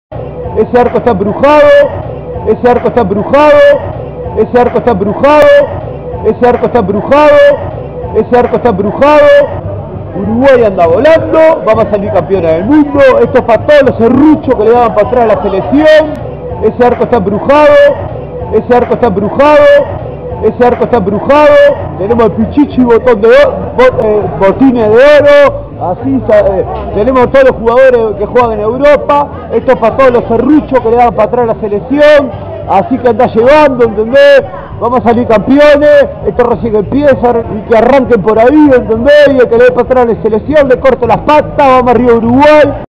"ese arco está embrujado"
"Ese arco está embrujado" como bien dijo el Cóndor, que hasta Suárez ataja en el equipo uruguayo en el encuentro de cuartos de final entre Uruguay y Ghana. En la serie desde el punto del penal, el golero uruguayo Muslera se erigió en el héroe al contener los disparos de Jonathan Mensah y Dominic Adiyiah. Uruguay avanzó a las semifinales de la Copa Mundial de la FIFA Sudáfrica 2010 al vencer a Ghana 4-2 en la definición desde los puntos del penal, luego de que el partido, jugado en el estadio Soccer City de Johannesburgo el 2 de julio, finalizara igualado 1-1.
Radio Vilardevoz en el 95.1 FM desde el Hospital Psiquiátrico Vilardebó en Millán 2515, Montevideo, Uruguay.